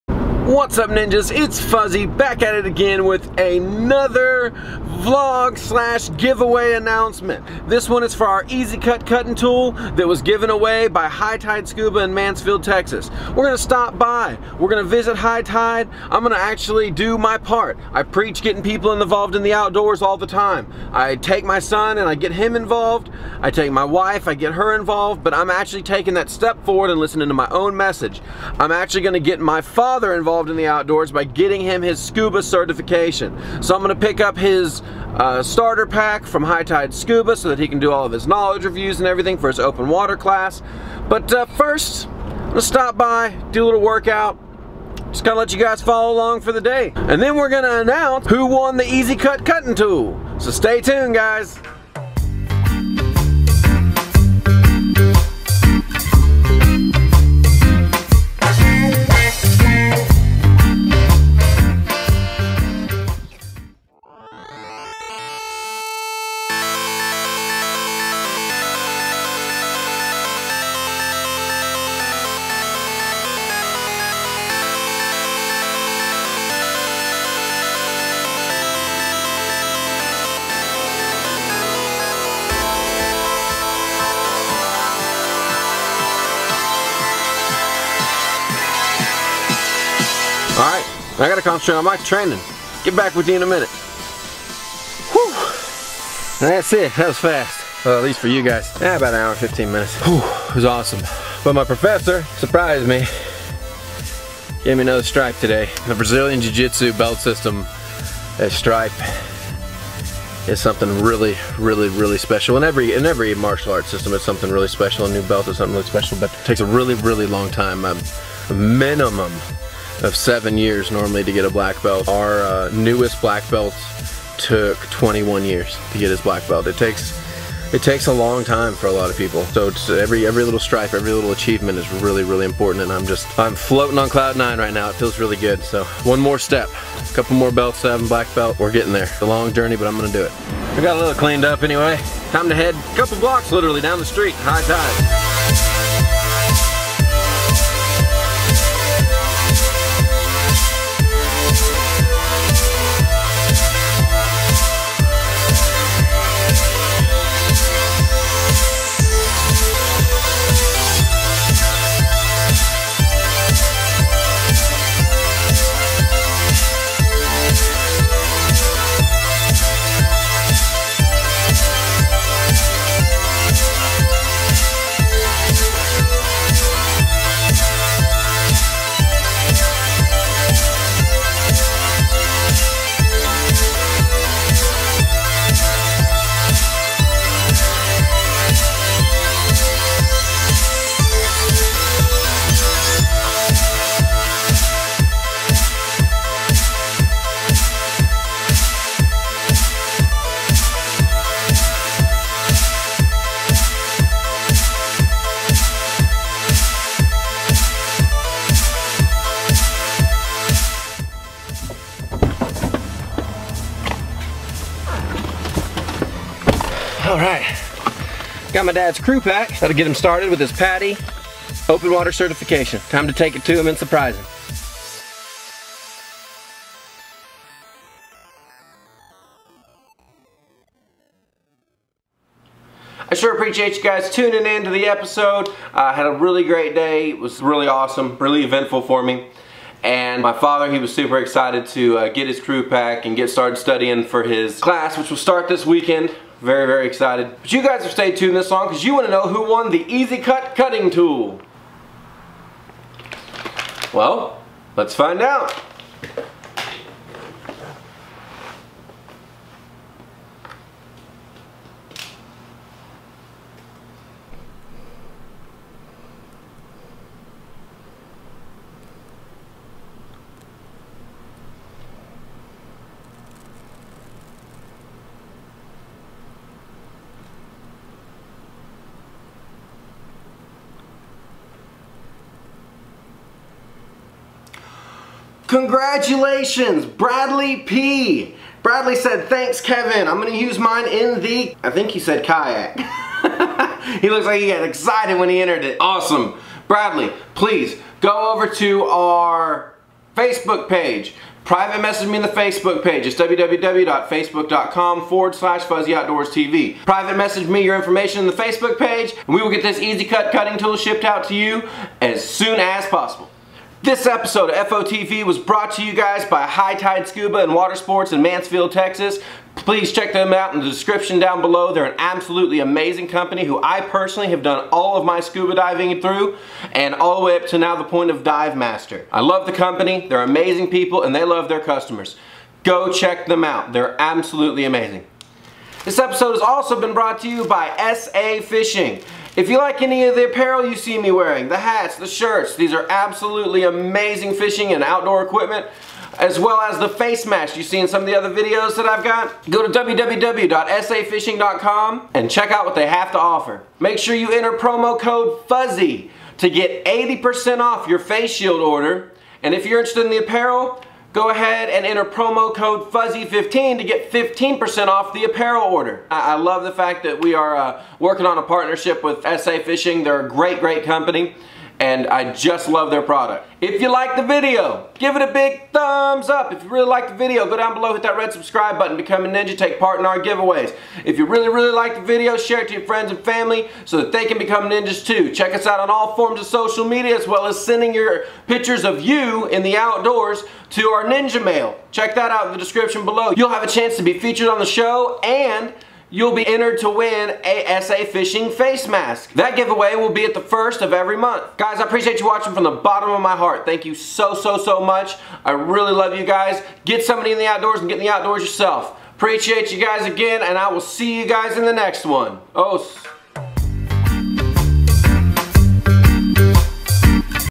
"EEZYCUT Cutting tool Giveaway announcement" FOTVLog #11 Fuzzy surprises his Dad
Fuzzy lets you follow along as he surprises his dad with a PADI scuba certification. He also announces the winner of the High Tide Scuba EEZYCUT Cutting tool giveaway.

High Tide Scuba
Kevin

Send your outdoor pics to us for a chance to be featured on the show at

Music legally downloaded for reuse from Digital Sound Promotion.